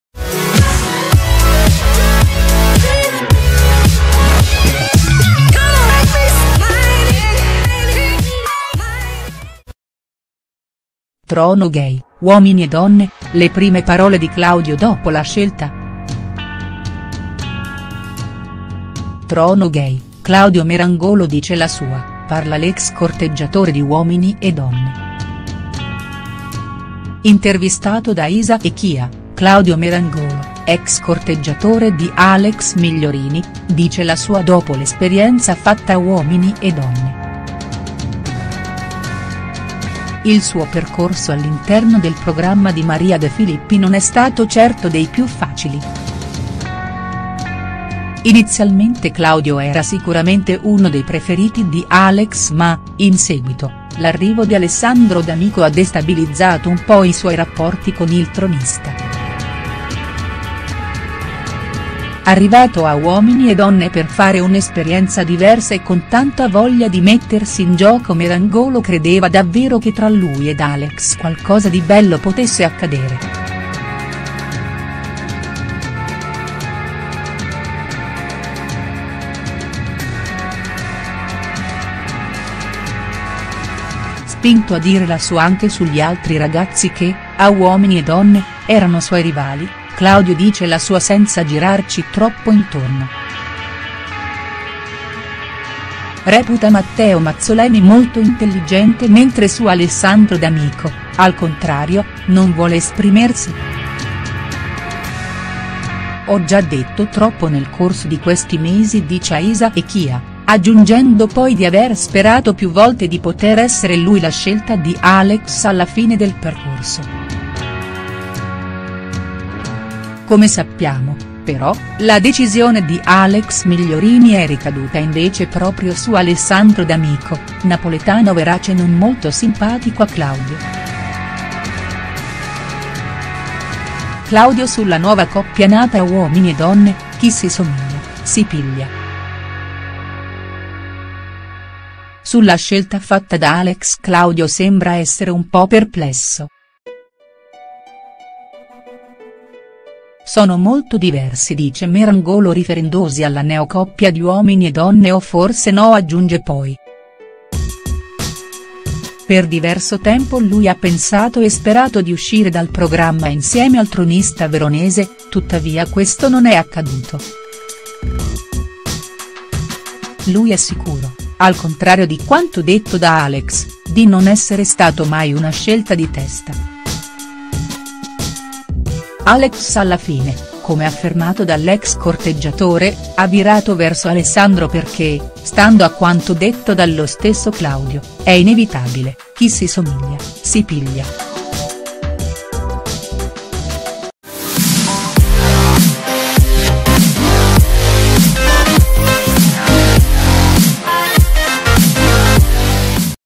Trono Gay, Uomini e Donne: le prime parole di Claudio dopo la scelta| STARS NEWS
Trono Gay, Claudio Merangolo dice la sua: l'ex corteggiatore, dopo la scelta, di Uomini e Donne parla di Alex Migliorini e Alessandro D'amico.